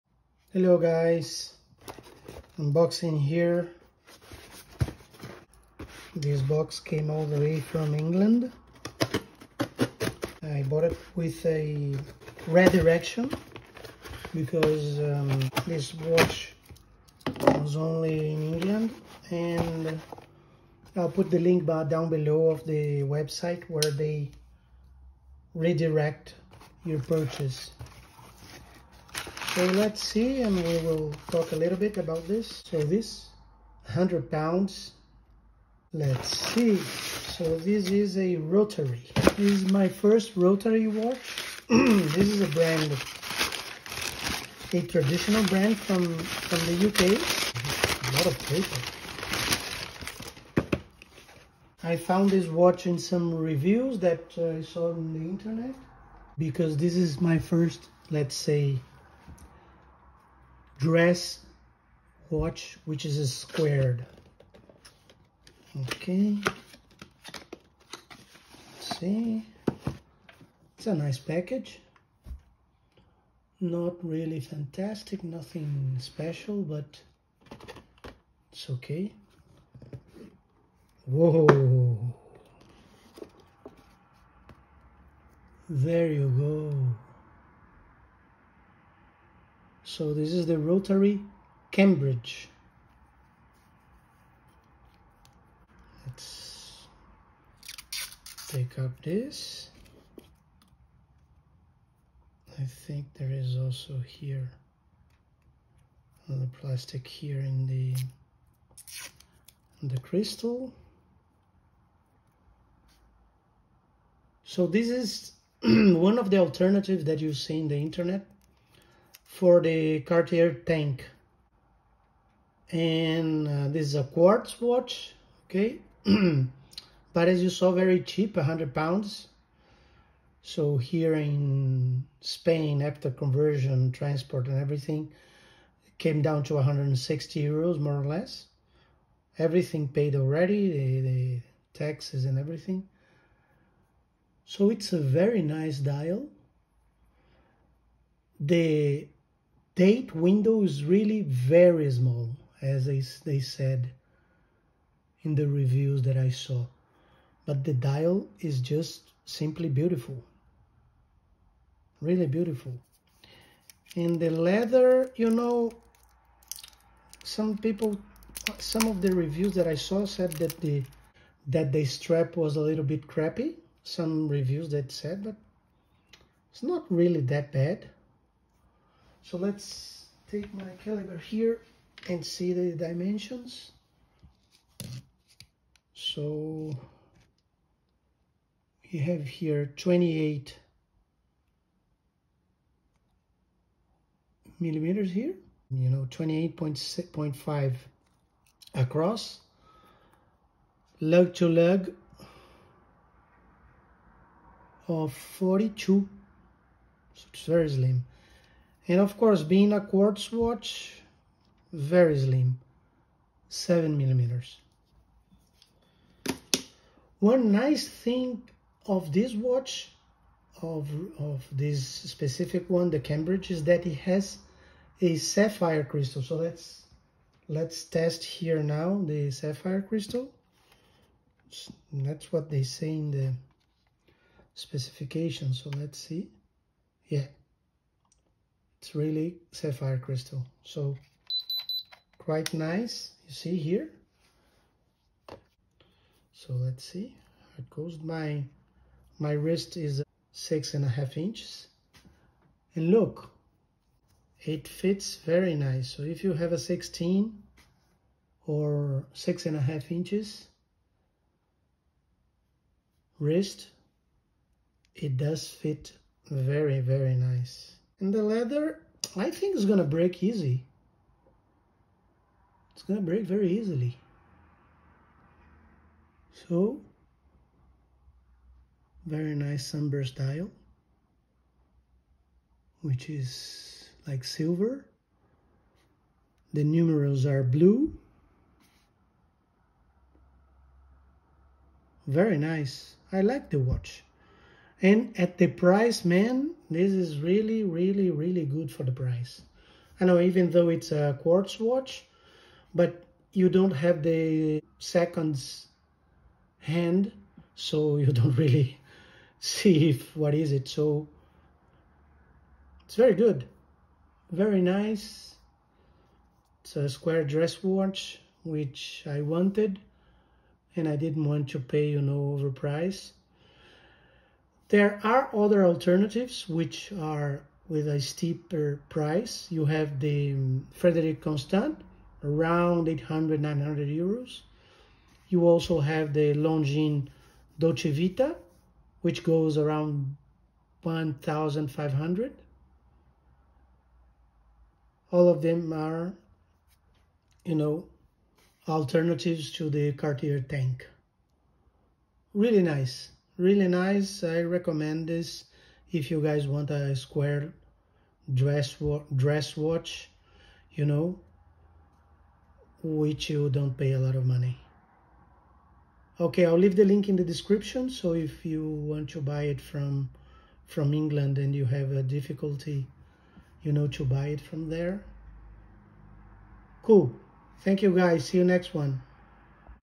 Rotary Cambridge unboxing and quick review
Delivery worked well to Spain, with all taxes and duties included, no customs work for me.